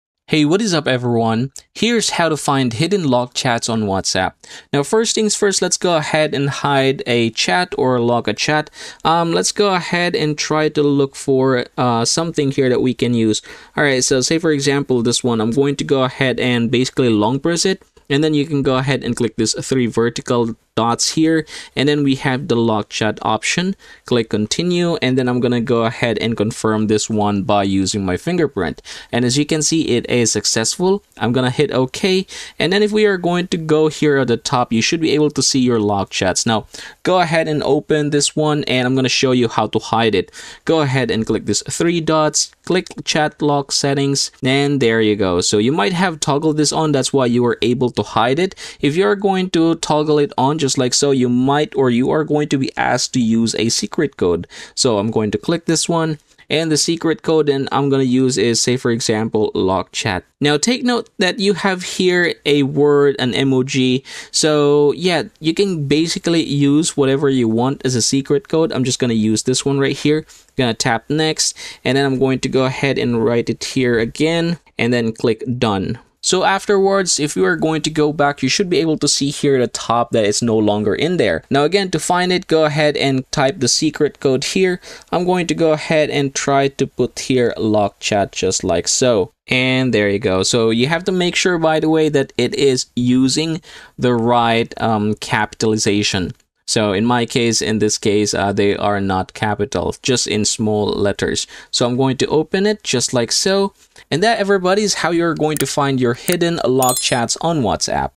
How To Find Hidden Locked Chats On WhatsApp
In this quick tutorial, you'll learn how to effortlessly access your locked chats, ensuring that your private messages remain secure yet accessible.

WhatsApp’s chat lock feature allows you to keep sensitive conversations hidden from prying eyes. Whether you want to secure personal messages or maintain confidentiality in group chats, this feature is essential for anyone who values privacy.